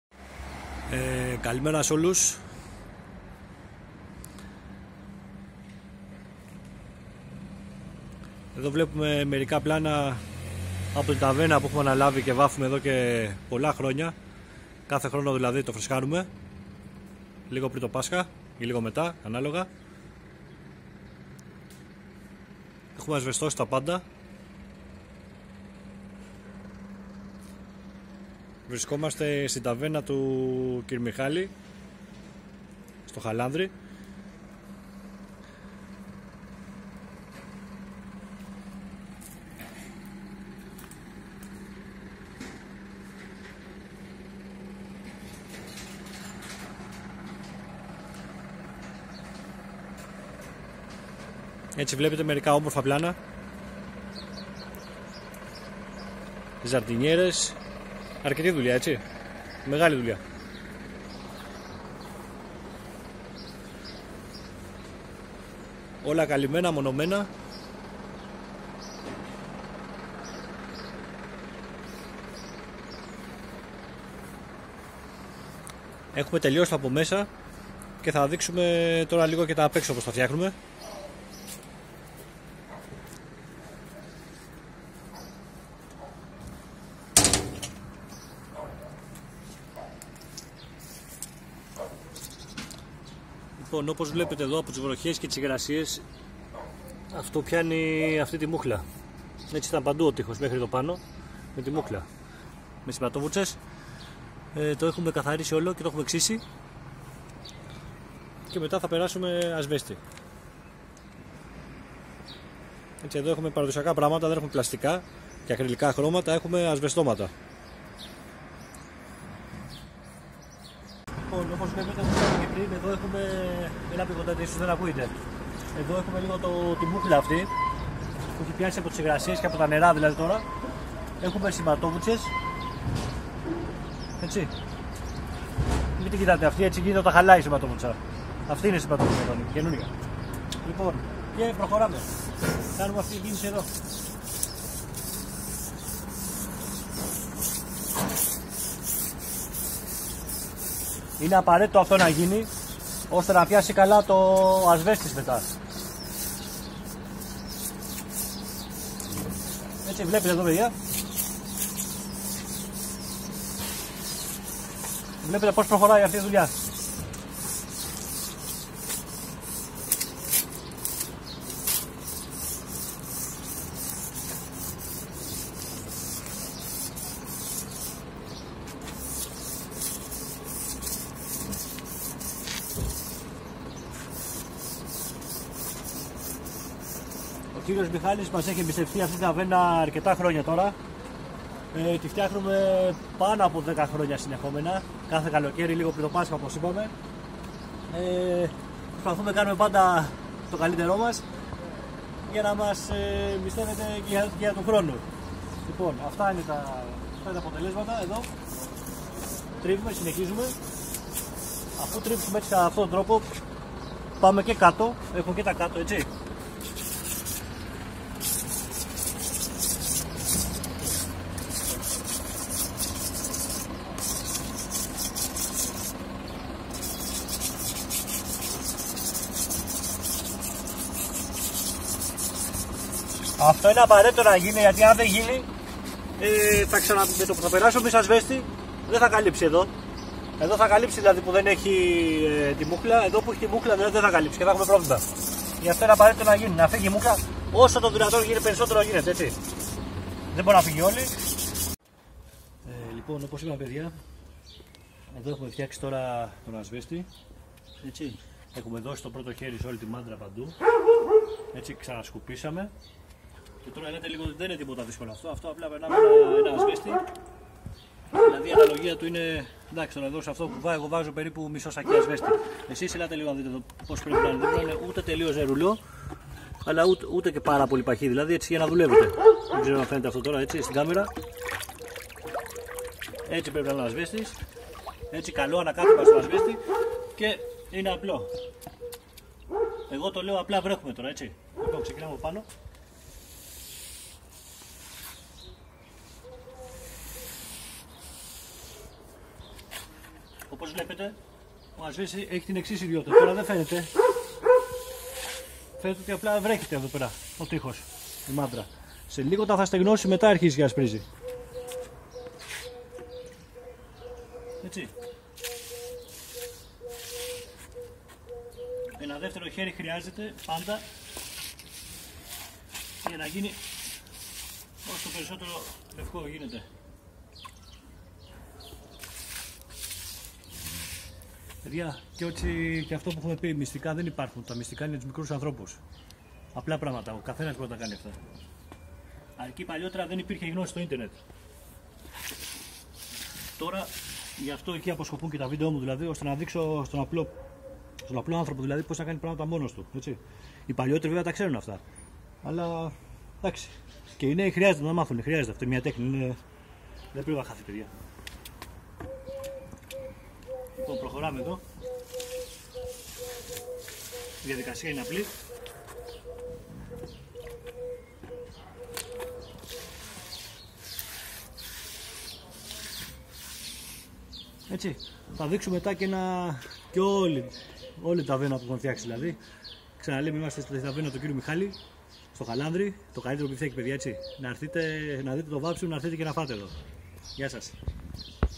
Βάψιμο σε ταβέρνα στο Χαλάνδρι με ασβέστη | Ασβέστωμα, άσπρισμα
εδω δειχνουμε πως μπορουμε να βαψουμε με ασβεστη μονοι μας χωρις ιδαιτερο κοπο. Είναι μια πολύ οικονομική και εύκολη λύση και η ποιότητα του λευκού είναι εξαιρετική. Σκουπίζουμε να φύγουν οι σκόνες και ότι άλλο ξένο σώμα υπάρχει στην επιφάνεια μας πλην του σοφά μας. Ακόμα εκεί που θα περάσουμε το υλικό μας πρέπει να είναι στεγνο.
Ο ασβέστης περνιέται με βούρτσα και όχι με ρολό. Δεν χρειάζεται κάποιο αστάρι πρώτα όμως θα χρειαστούν 2 ή 3 χέρια για να έχουμε το τέλειο αποτέλεσμα. Αγοράζουμε την ποσότητα που θα χρειαστούμε σε σακούλες ασβέστη από κάποια μάντρα οικοδομικών υλικών. Έπειτα παίρνουμε έναν κουβά ρίχνουμε τον ασβέστη μέσα και αραιώνουμε με νερό. Το βάψιμο πρέπει να γίνει σε ηλιόλουστες μέρες για να γίνει γρήγορα το στέγνωμα και να προχωρήσει η όλη διαδικασία. Εννοείται πως αν βρέξει ενώ βάφουμε θα μας καταστρέψει ότι έχουμε κάνει μέχρι τότε.
Βαψίματα με τον Ιούλιο